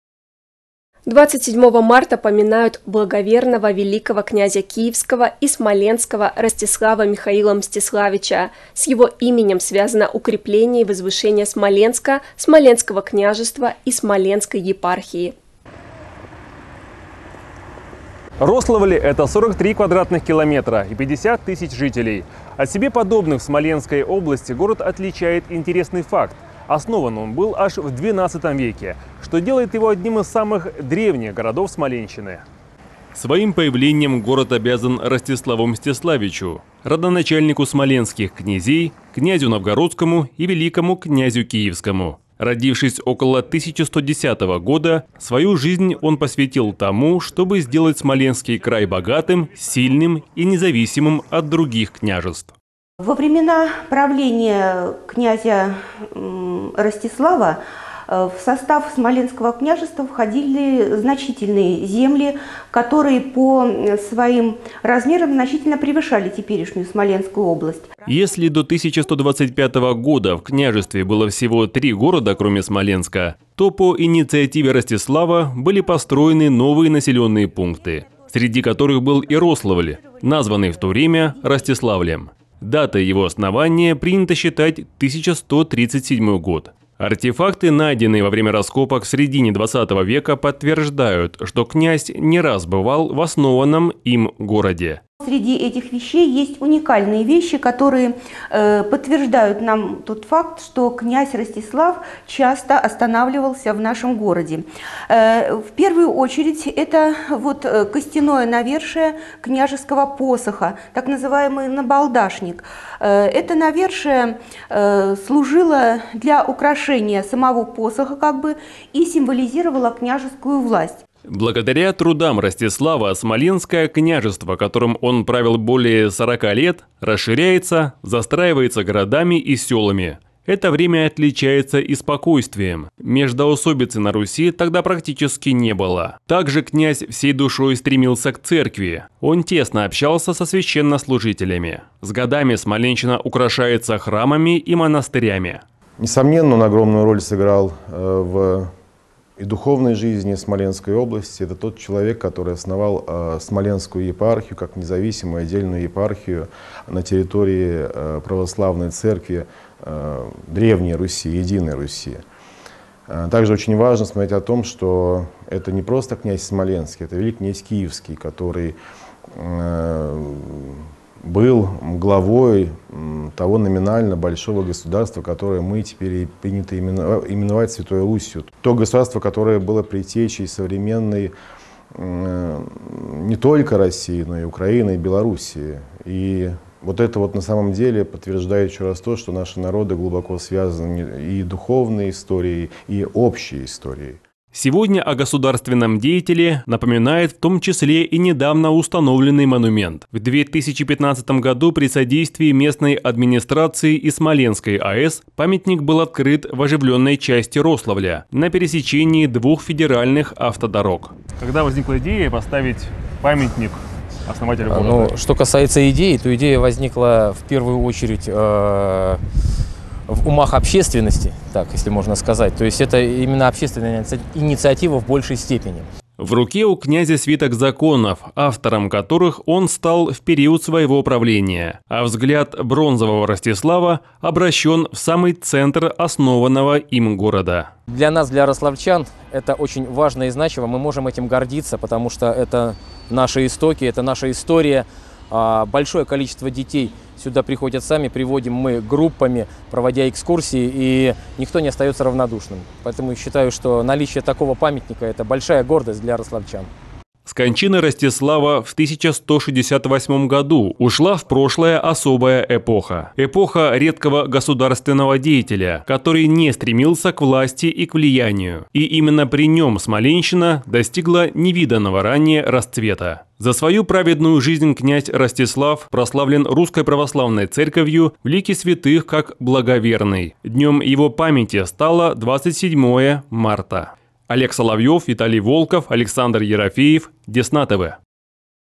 Десна-ТВ: День памяти Благоверного князя Ростислава
27 марта поминают благоверного великого князя Киевского и Смоленского Ростислава-Михаила Мстиславича. С его именем связано укрепление и возвышение Смоленска, Смоленского княжества и Смоленской епархии.